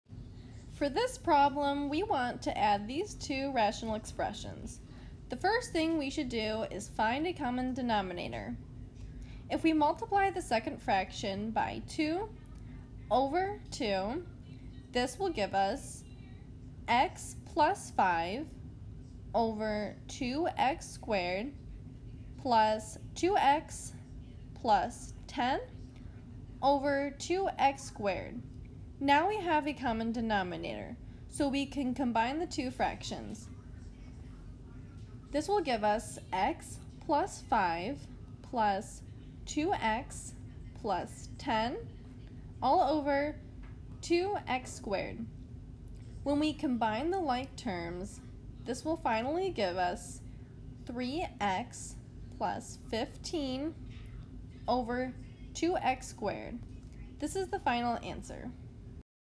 Rational Expressions: Add (x + 5)/2x^2 + (x + 5)/x^2
Erin from SVSU Micro Math helps you add rational expressions and simplify if possible.  A rational expression is a fraction of polynomials.  To add fractions, we first find a common denominator.

Problem: Add (x + 5)/2x^2 + (x + 5)/x^2